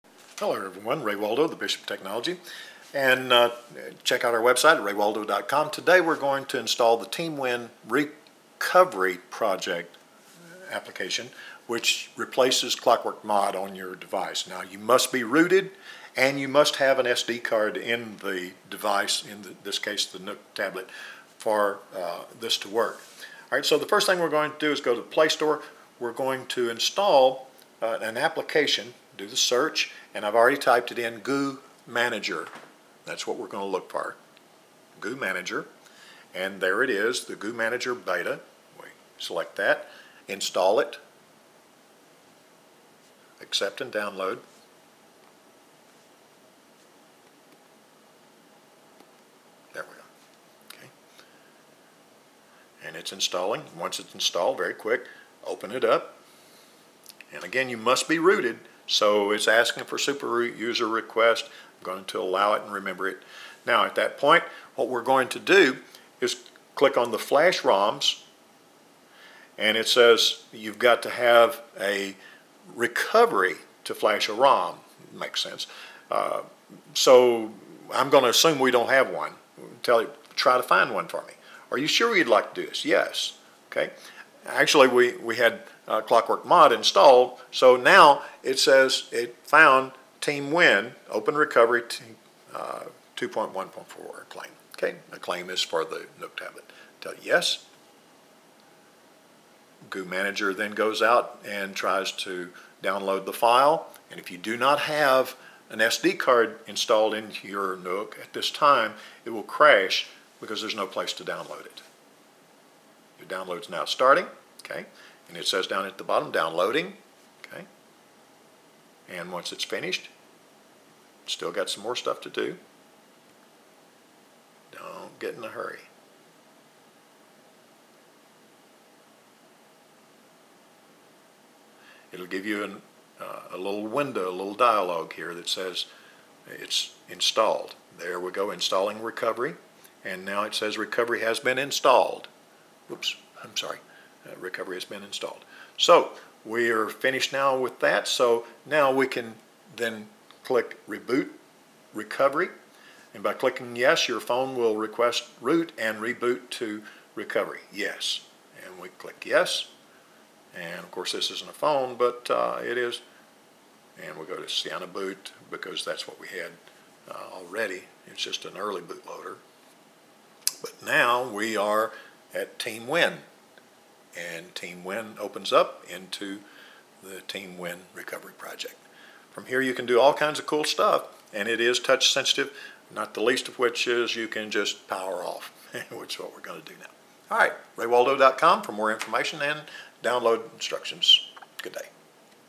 Install TWRP on the Nook Tablet (requires ROOT)
If you have rooted your Nook Tablet (8/16gb), then this simple step-by-step guide will show you how to replace your present recovery (either CWM or stock) with the Team Win (touch sensitive) Recovery Project. All done from the User Interface.